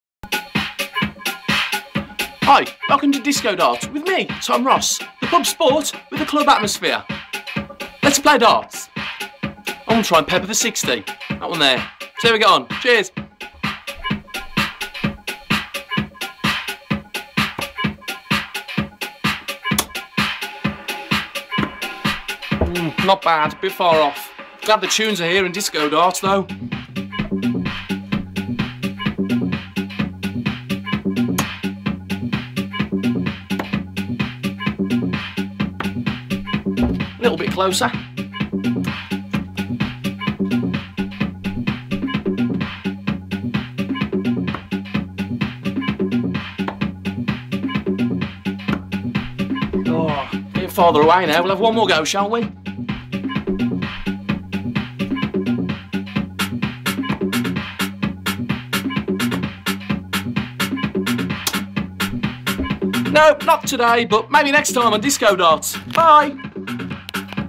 Disco Darts
Welcome to the world of disco darts, the pub game with the club atmosphere!